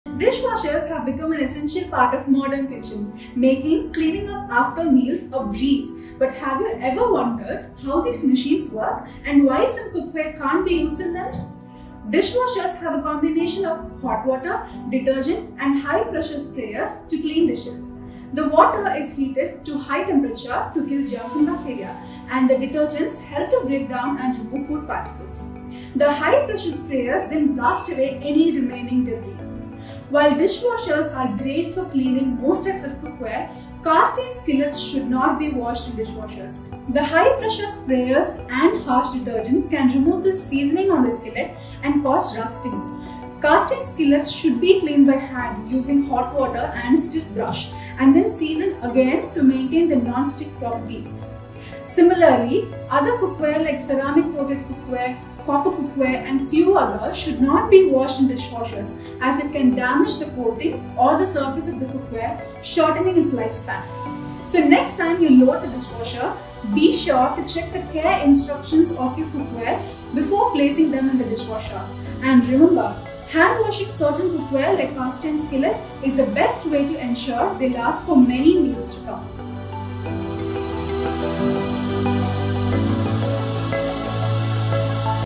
How dishwashers work & why certain products are not dishwasher safe ? | The Indus Valley
Dishwashers have revolutionized the way we clean up after meals, but have you ever wondered how they work and why some of your cookware can't go in them? 🔍
Watch the video to find out why certain types of cookware shouldn't be washed in the dishwasher, and how to care for them properly. 👀